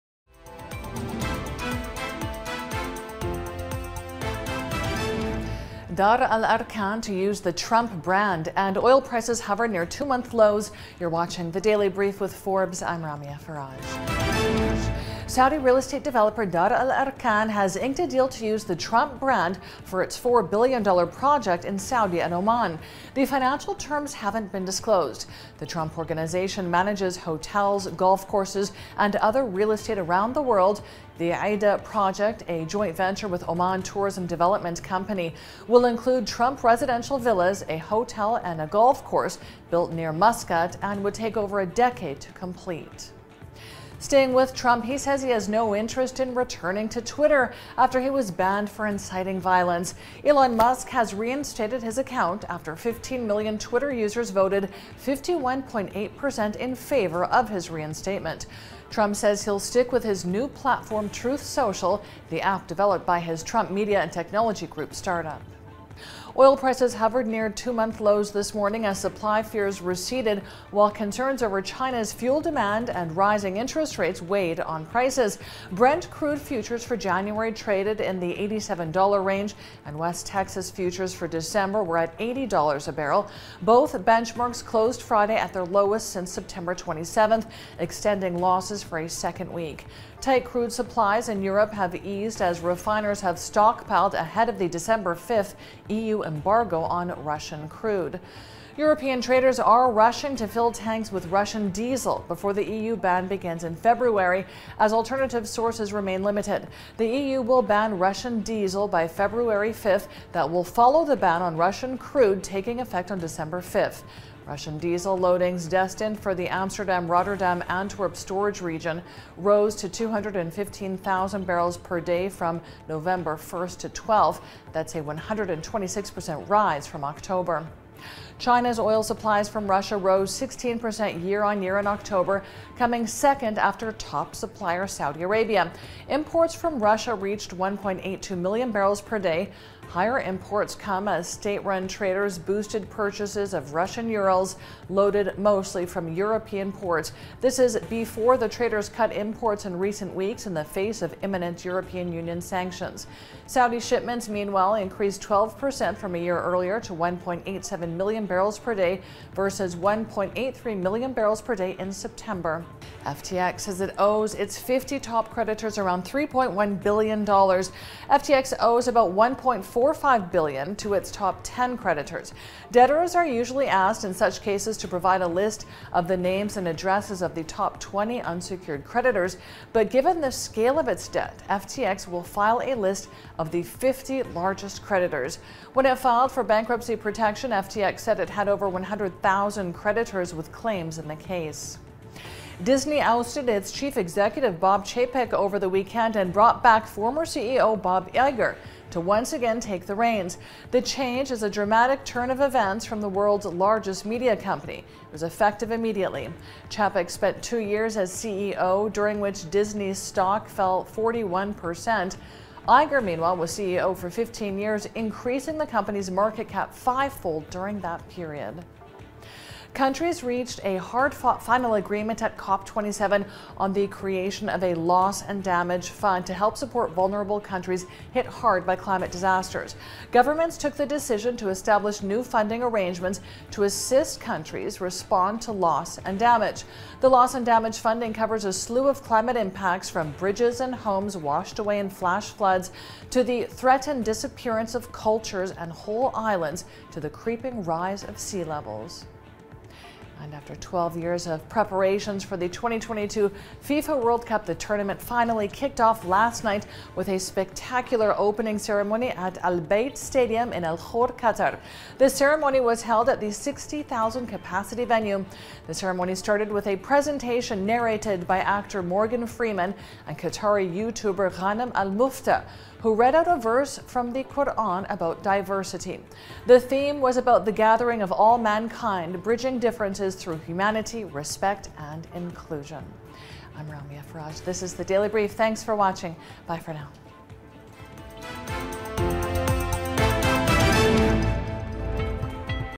Midday Brief 21.11.2022
Dar Al Arkan to use the Trump brand & oil prices hover near two-month lows, on the DailyBrief with @RamiaFarrage